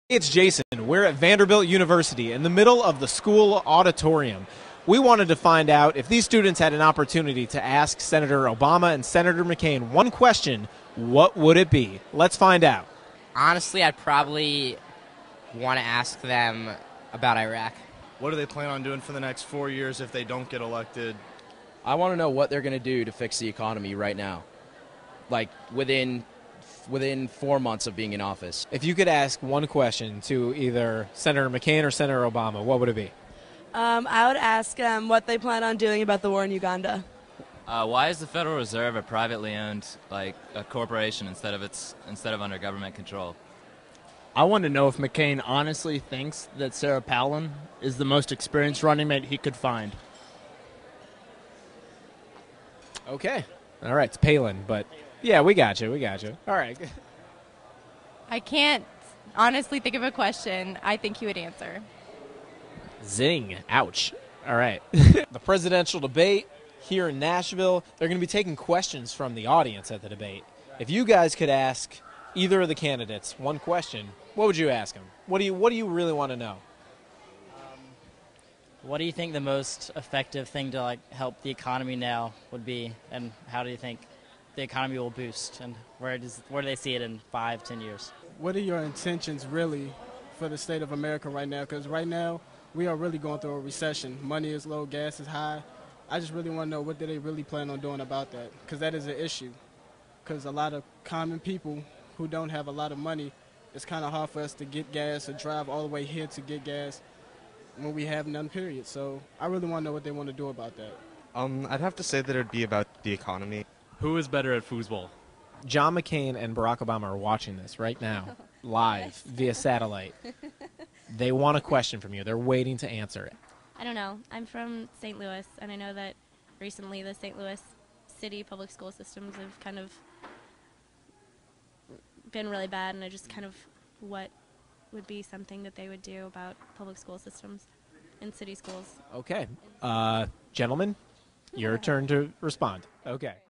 If you could ask the candidates a question ...
Youth Vote '08 correspondents Jason Zone Fisher and Gaby Roman talk to Nashville students about what question they'd ask the candidates if they were at the town hall debate.